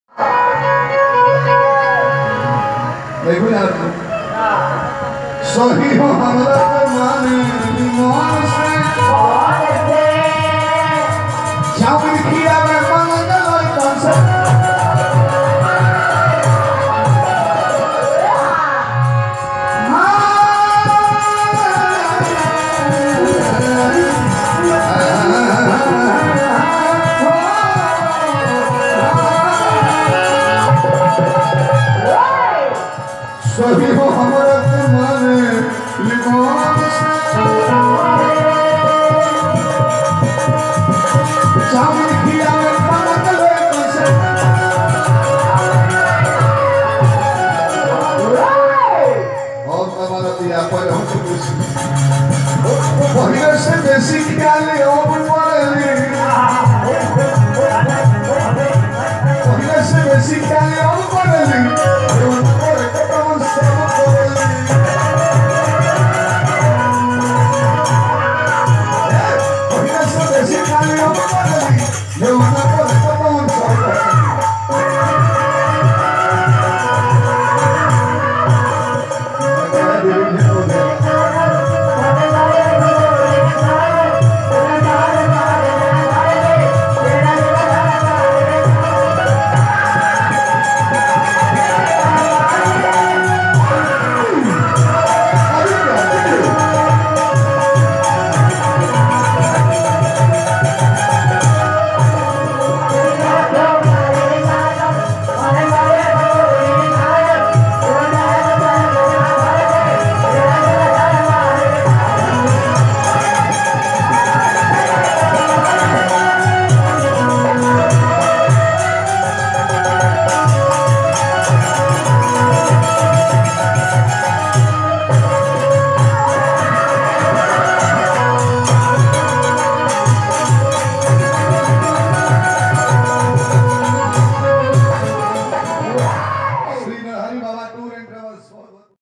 सुनिए अरविंद अभियंता क्या बोल दिए Shivnarayan Yadav को देखिए महा मुकाबला Stage Programme Dugola
Arvind Aviyanta Or Shivnarayan Yadav ka New Dugola Muqabala Stage Programme 2019 Baliya Me Is Programme Ko Ek Bar Jarur Dekhiye Acha Lage To Video Ko Like Kare  And Channel Ko Subscribe Kare Thank You